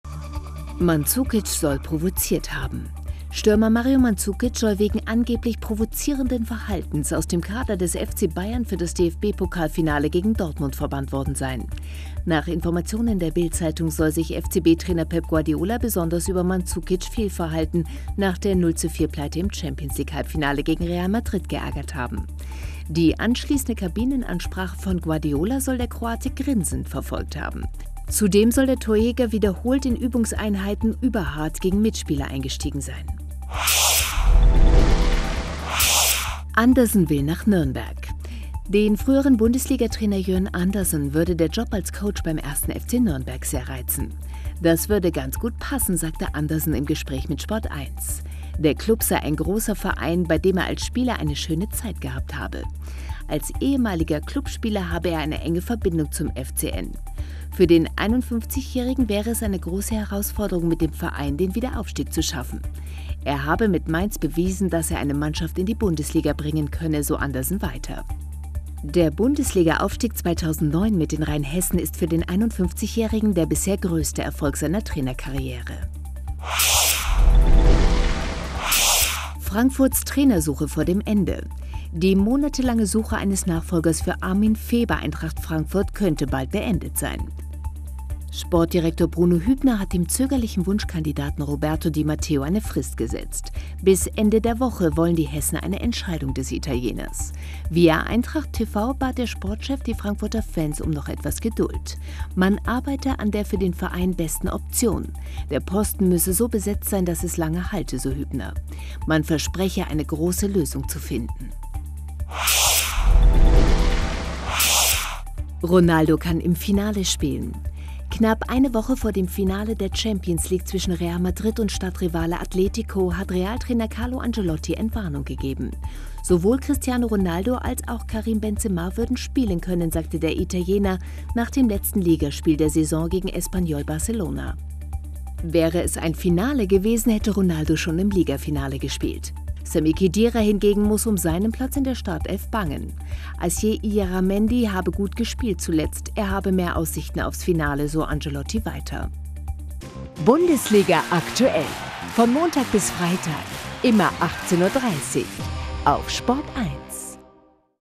Mandzukic soll Pep provoziert haben - Andersen will nach Nürnberg | SPORT1 NEWS
1. Mandzukic soll Pep provoziert haben
2. Andersen will nach Nürnberg
3. Frankfurts Trainersuche vor dem Ende
4. Ronaldo fit fürs Finale